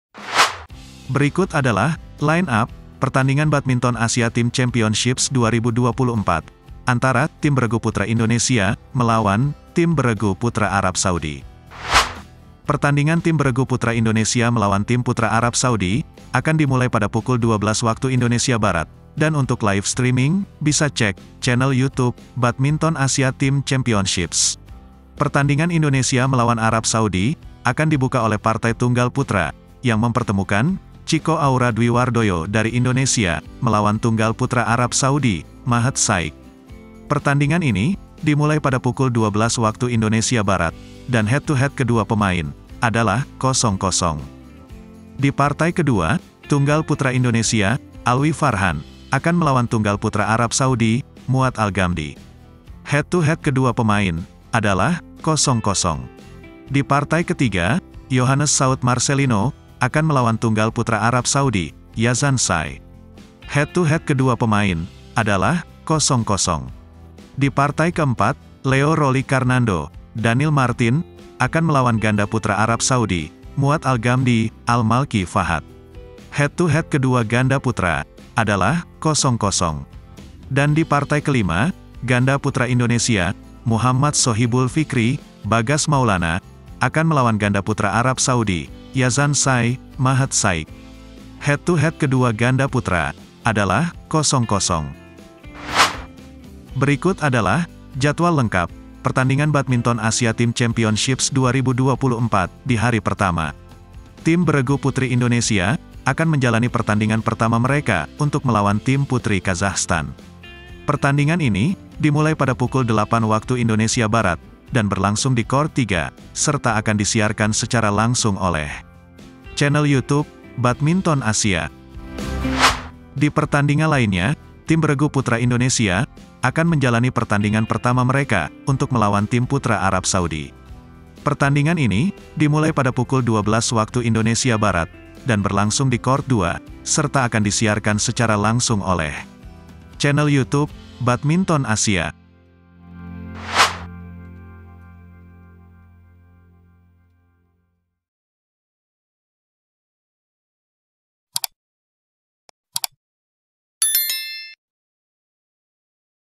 Line Up Indonesia Vs Arab Saudi Badminton Asia Team Championships 2024 Hari Ini Live Pukul 12:00 WIB
Line Up Indonesia Vs Arab Saudi Badminton Asia Team Championships 2024 Hari Ini Live Pukul 12:00 WIB Di Channel Youtube Badminton Asia Team Championships 2024